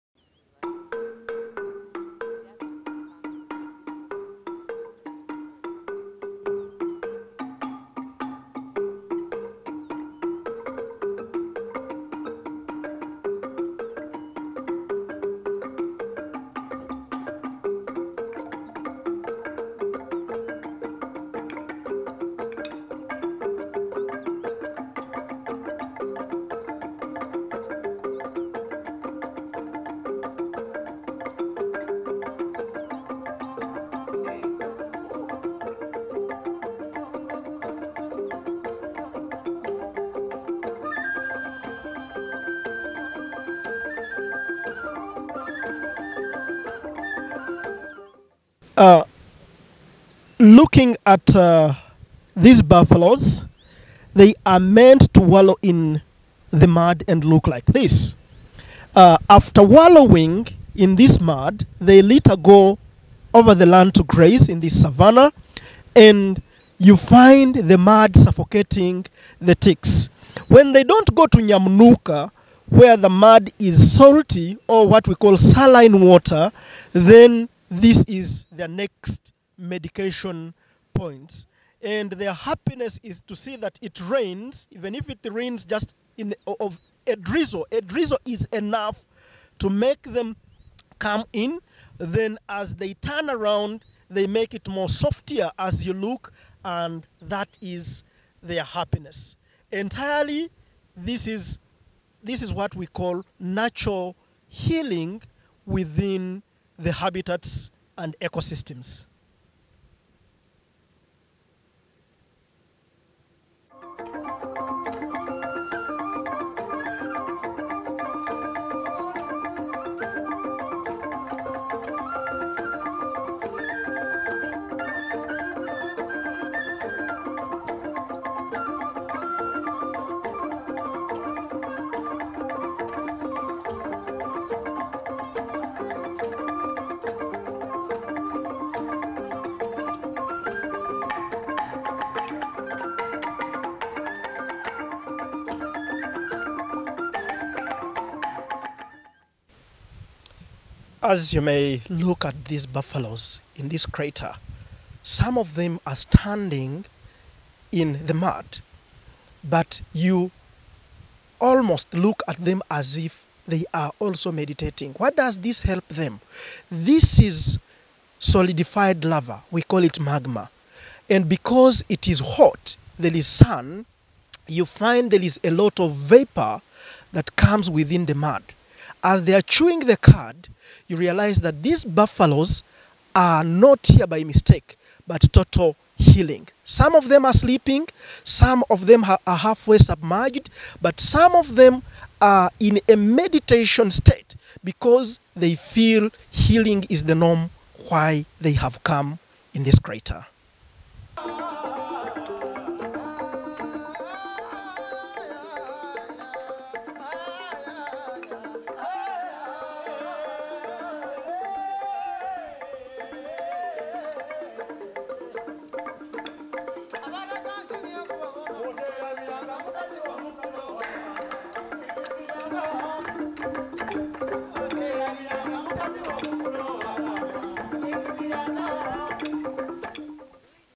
WHY BUFFALO WALLOW IN MUD
Wallowing is a very important way for the buffaloes to maintain their body temperature.